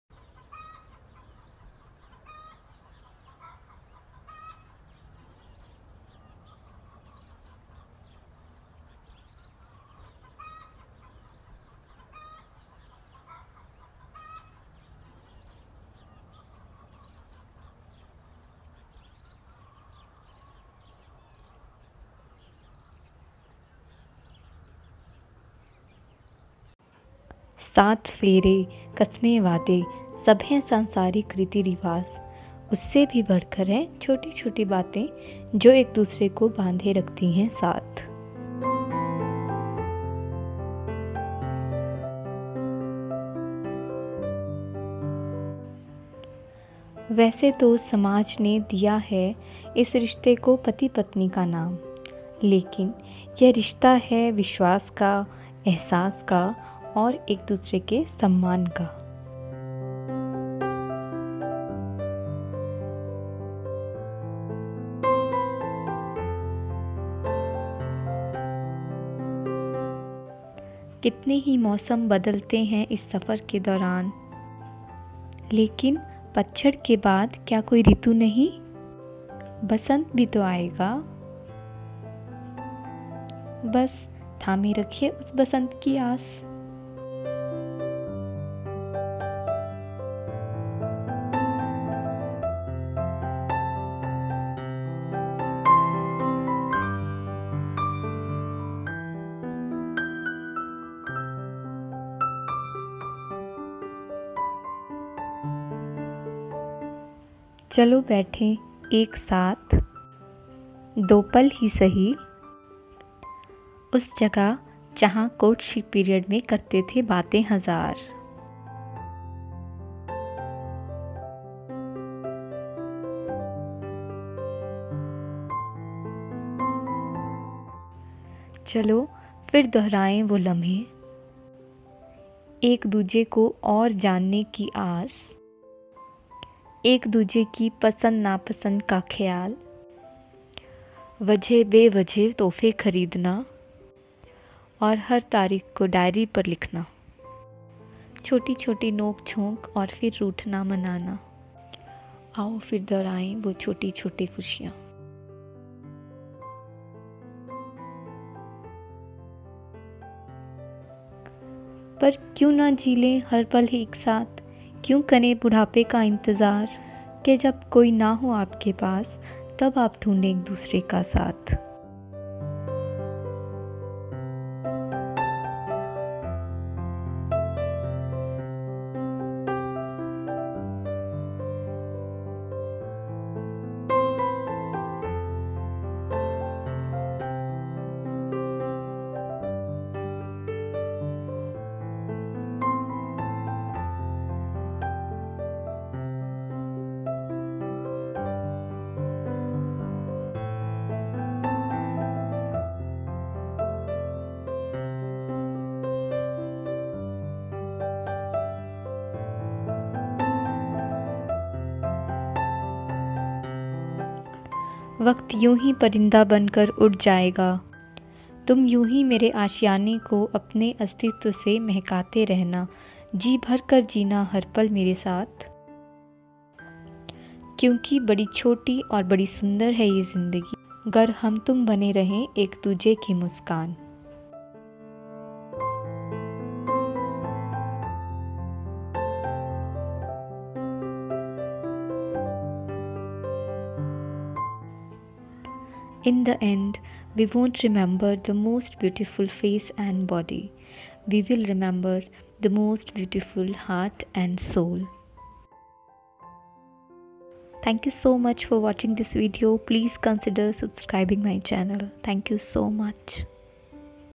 GROWING OLD WITH SOMEONE YOU LOVE
Hello friends
I hope u like this video and correlate this with your courtship period..
I have tried to show my feelings..

If u find anything matches with your life, please comment in comments section...

Thanks a lot for reading this.. ☺️